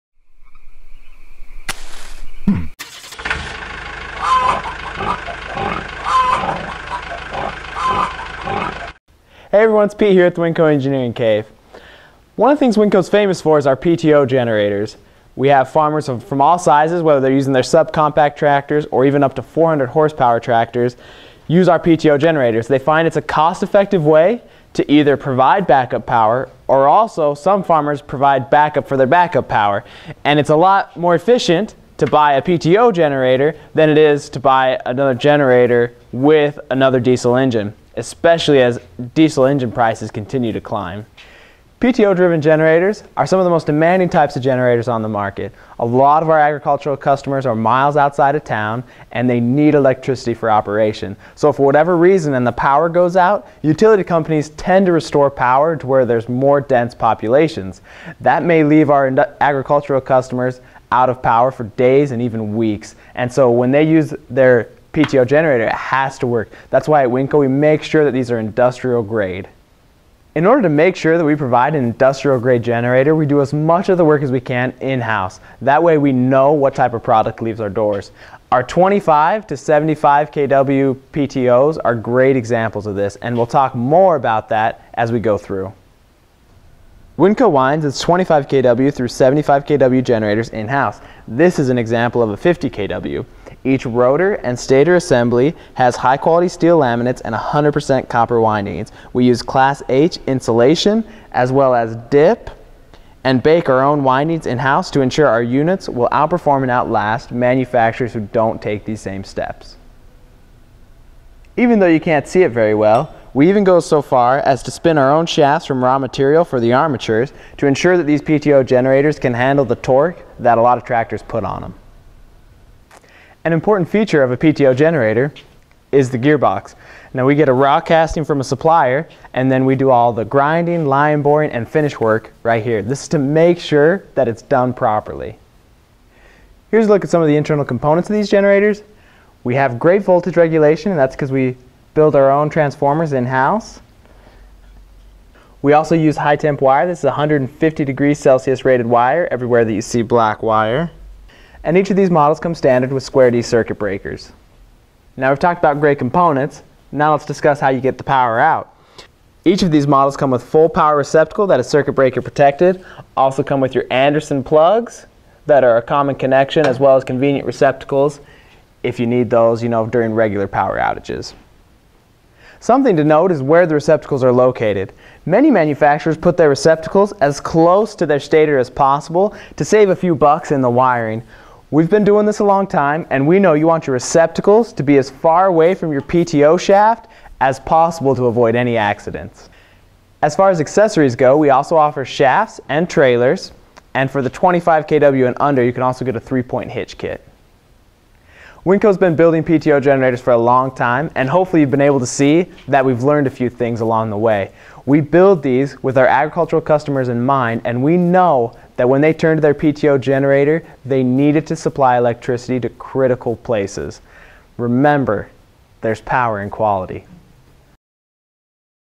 Winco Power Take-Off (PTO) Generators 25-75Kw
Shows off an example of our PTO generators that have strong roots in American Agriculture. Our PTO's are great for backup power or for backup to your backup power.